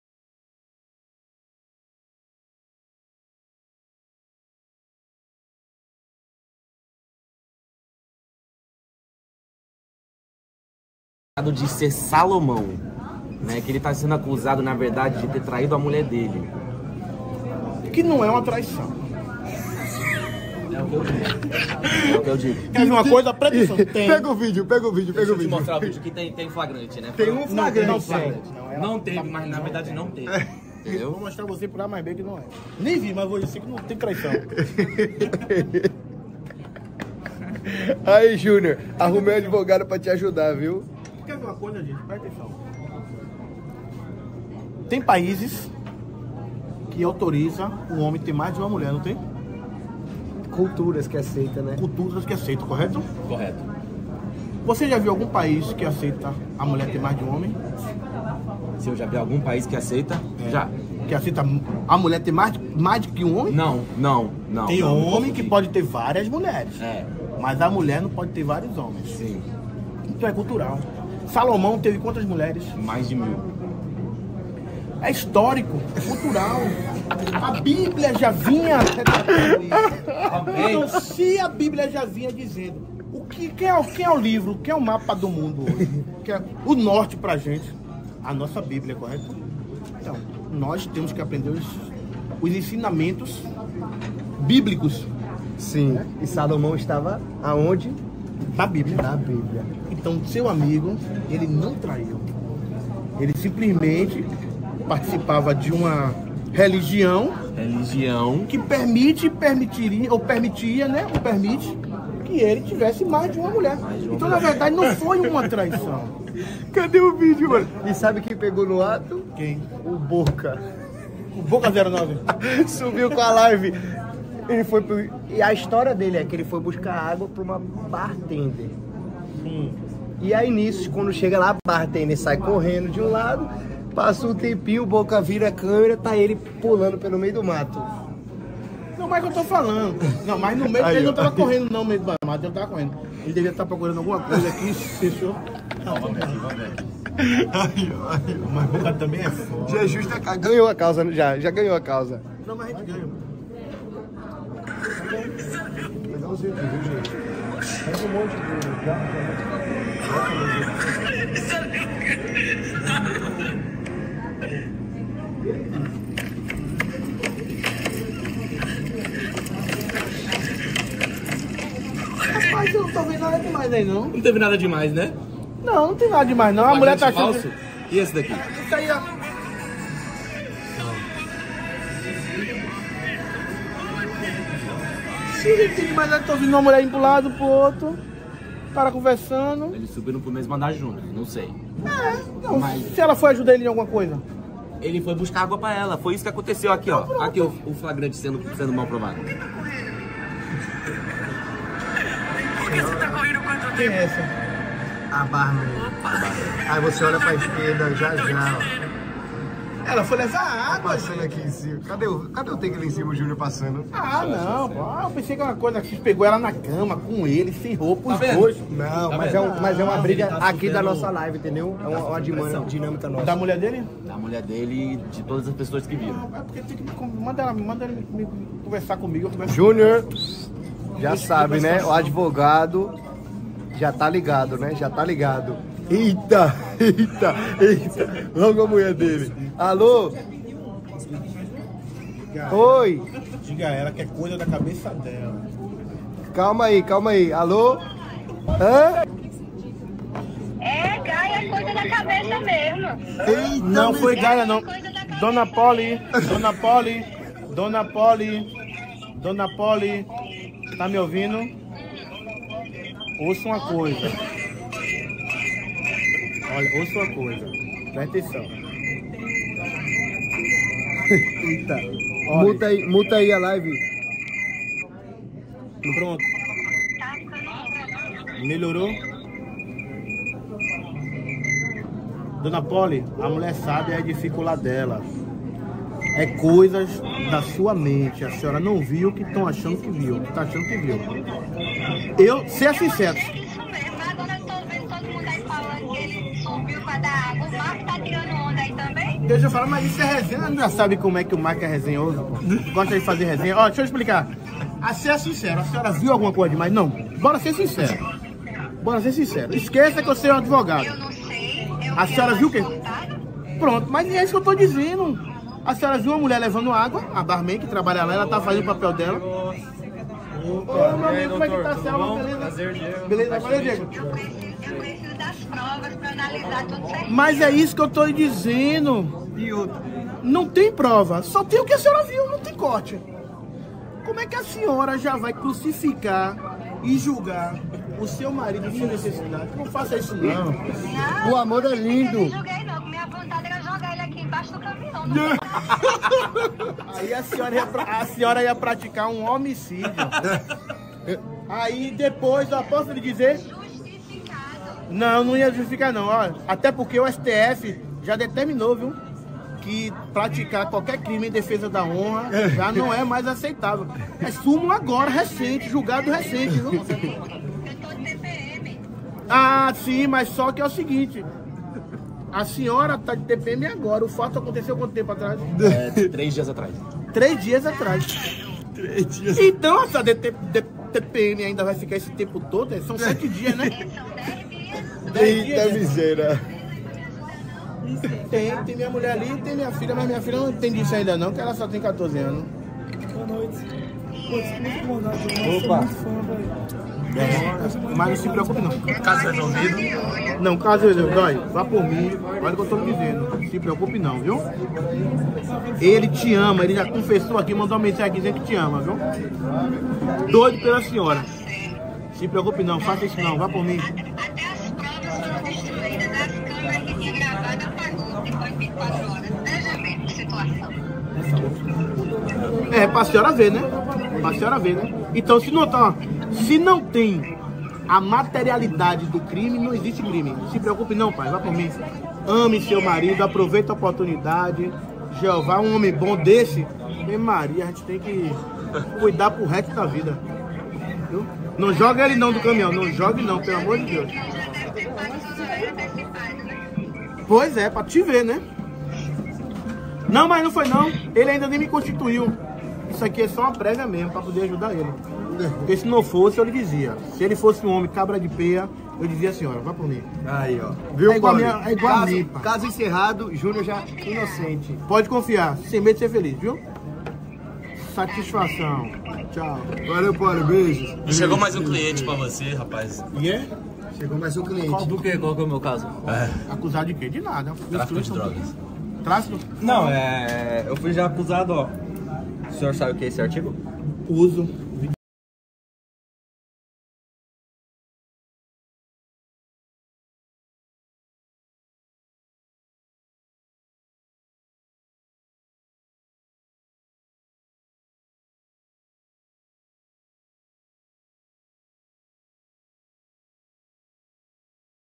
JON VLOGS PERGUNTA SOBRE JUNIOR SALOMAO PARA DR JOÃO NETO - MULHER DO JUNIOR LIGA NA LIVE KKK
JonVlogs

jon vlogs
bonde do jon
maumau zk
jon
nzao
boca de 09
cabritoz
bdj
racha
hudson amorim